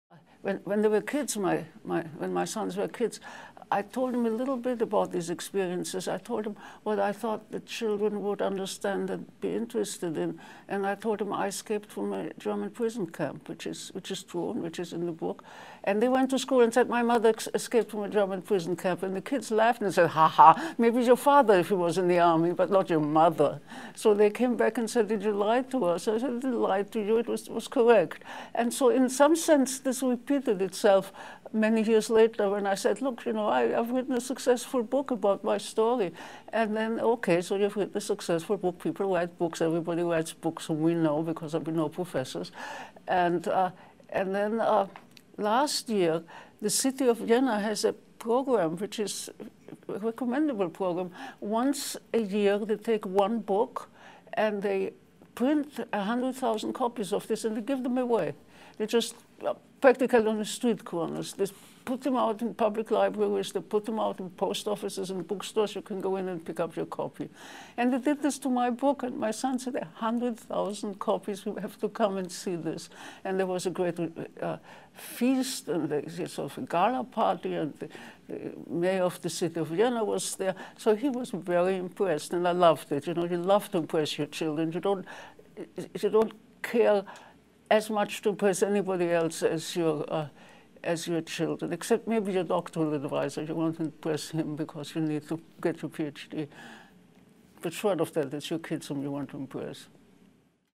UC Irvine's Ruth Kluger Discusses Her Holocaust Memoir
Ruth Kluger, UC Irvine German professor emerita, talks about her memoir, "weiter leben: Eine Jugend (To Continue to Live: A Childhood)," an account of growing up in Nazi-occupied Austria that has touched millions. Video by Kerrin Piche Serna, University Communications.